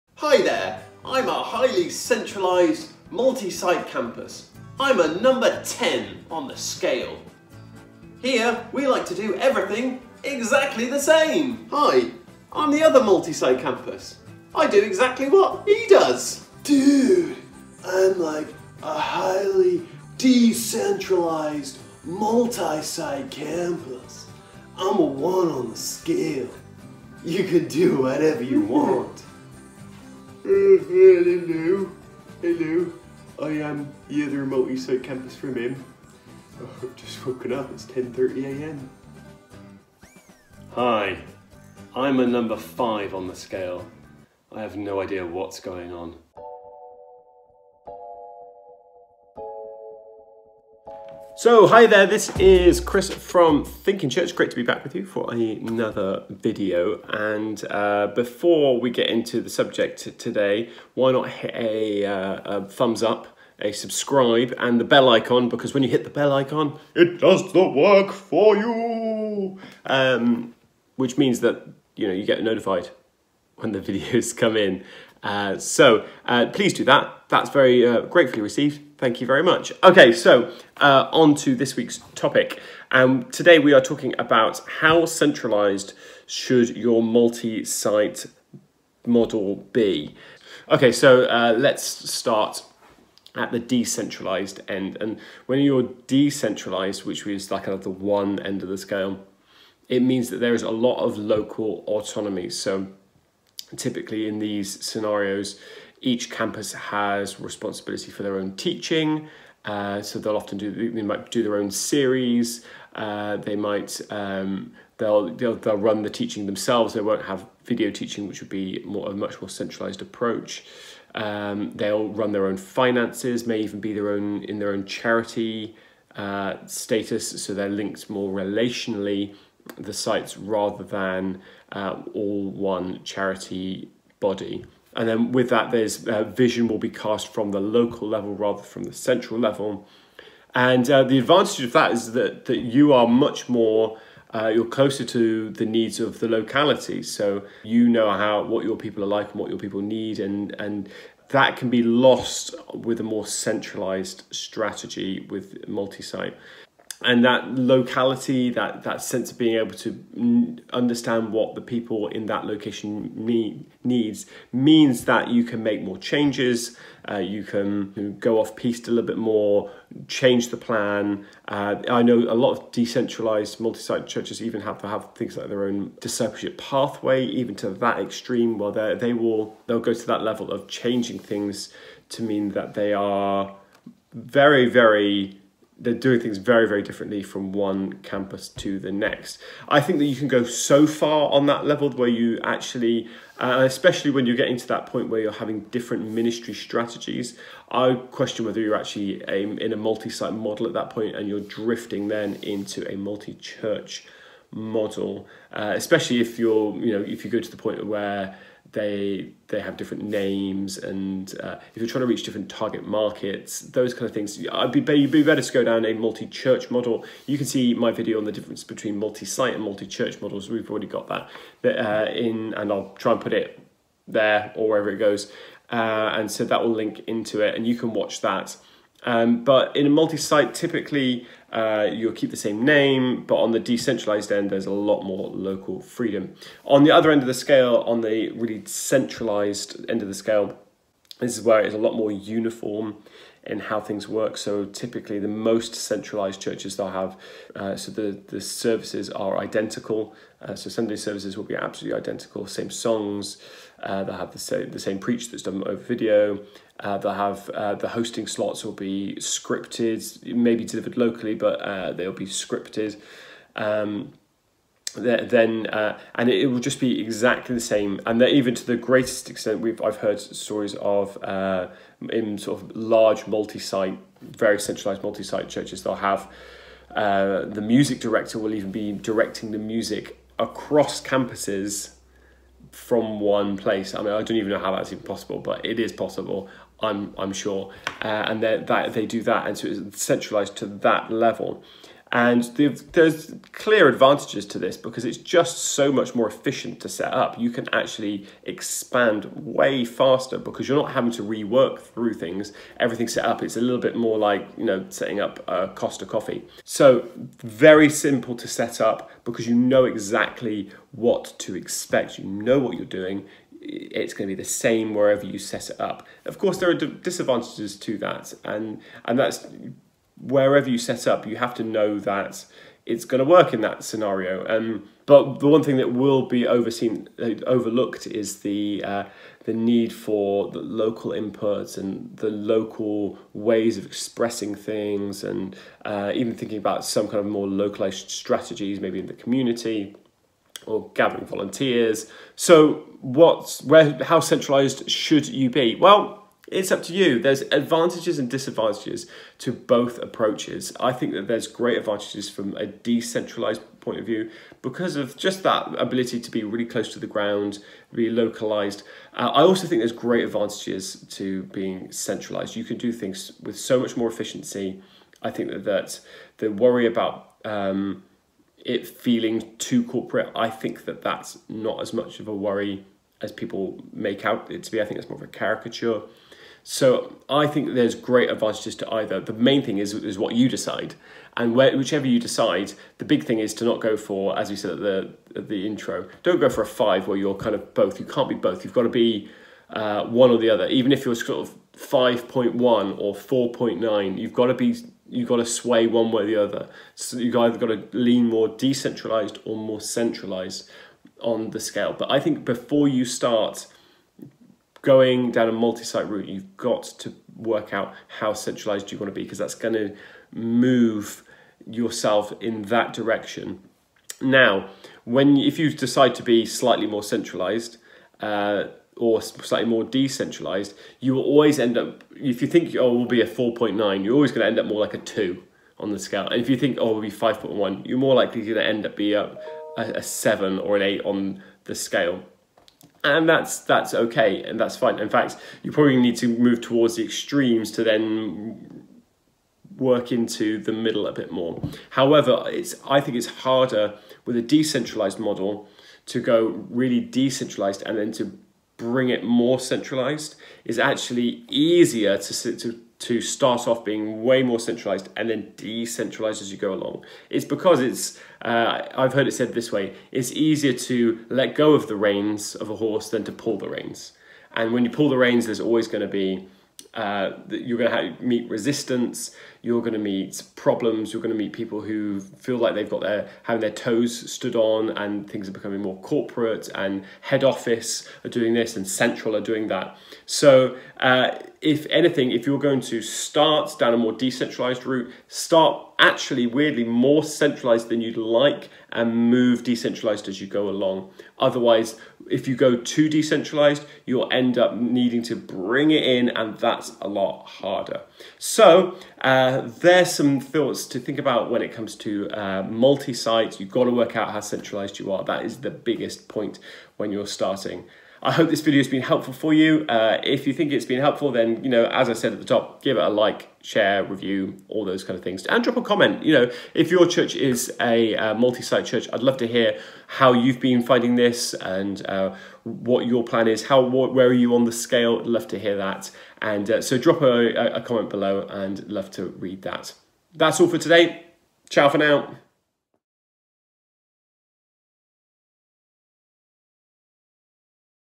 How centralised should you make your multisite strategy?
There's more than one way to do multisite, and you can fit anywhere on the sliding scale of centralisation. In this video, Chris explains how to think about  how centralised your multisite strategy should be.